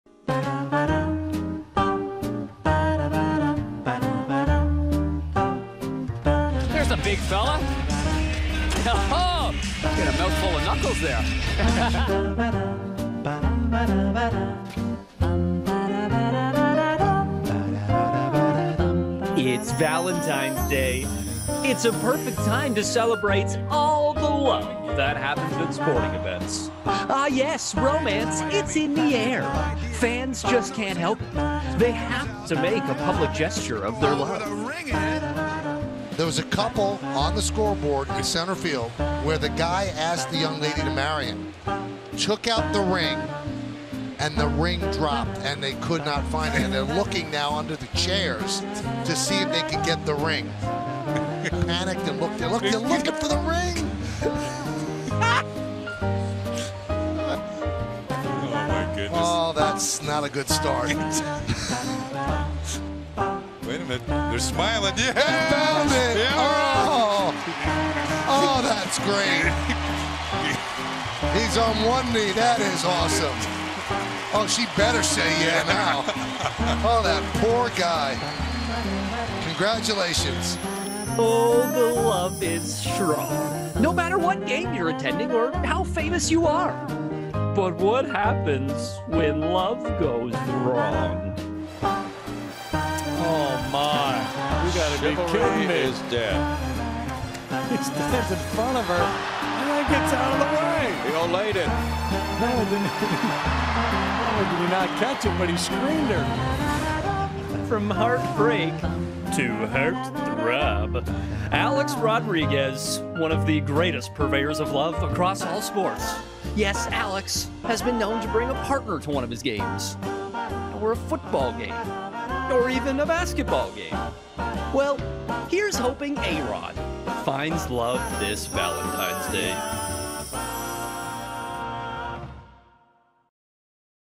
Valentines Day Flashback: Funniest Fan Moments
Sportsnet takes a look back at some of the best and funniest romantic moments from fans at sporting events.